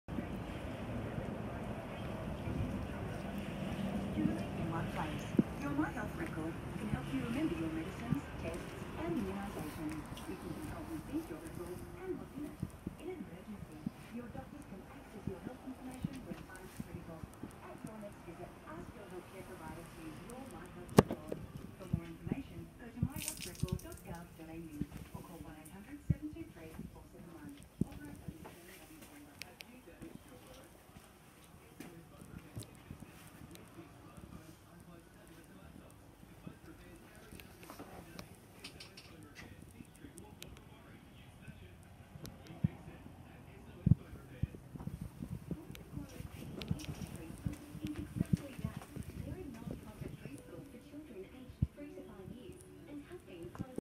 2019 JAYCO SILVERLINE 21.65.4 OUTBACK Watsons Caravans Port Macquarie (Stock #9539)
2019 JAYCO SILVERLINE 21.65.4 OUTBACK
Options Fitted:
Dual 100ah Batteries
Dual 150w Flexi Solar Panels
Gas Heater
ATM Increased to 600 kgs
TARE: 2783kg                 BALL: 193kg